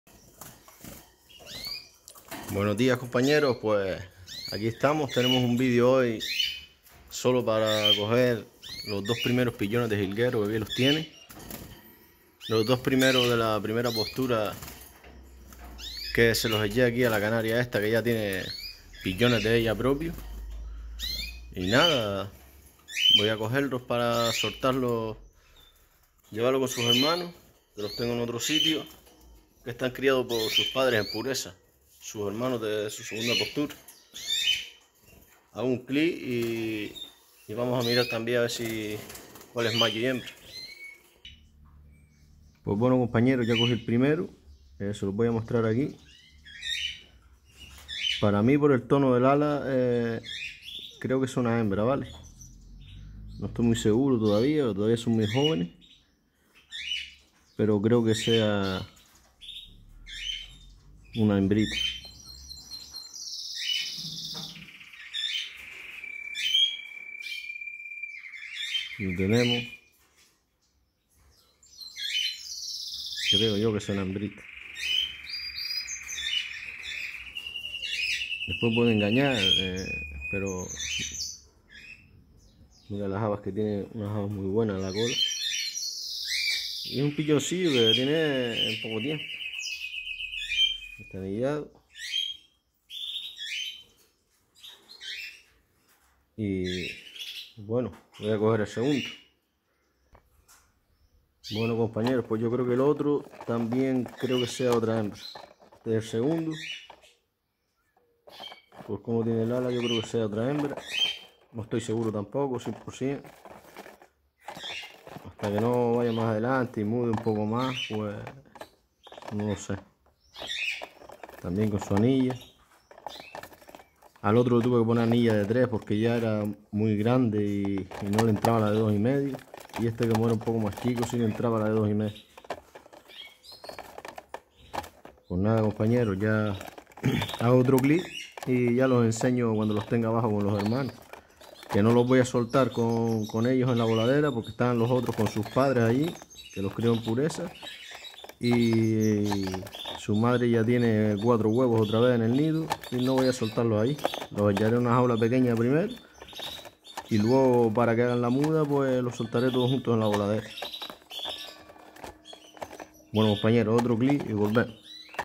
como van los 2 primeros pichones de la temporada aver si son machos o hembras?
dejen sus opiniones me sirven de mucha ayuda